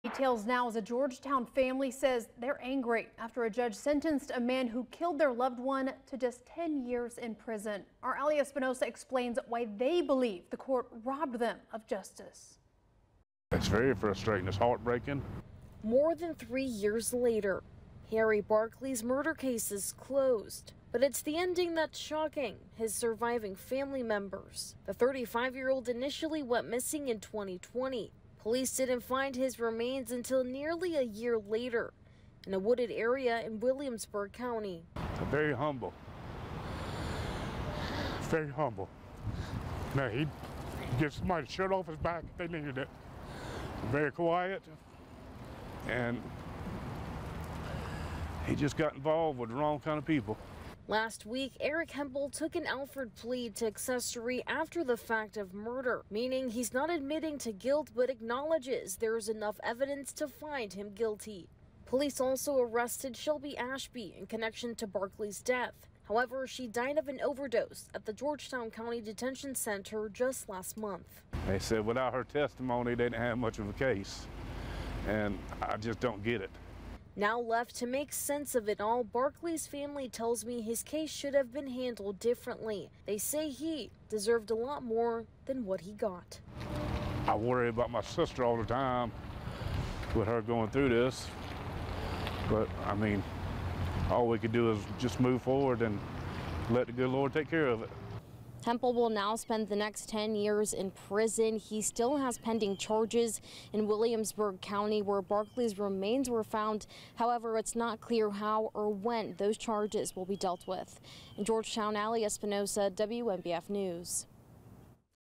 'It's heartbreaking': Family of slain Georgetown man calling for harsher penalty after killer sen...
5 year-old Harry Barkley’s family claim justice was not served amid a recent plea deal between the 15th Circuit Solicitor’s Office and Barkley’s alleged killer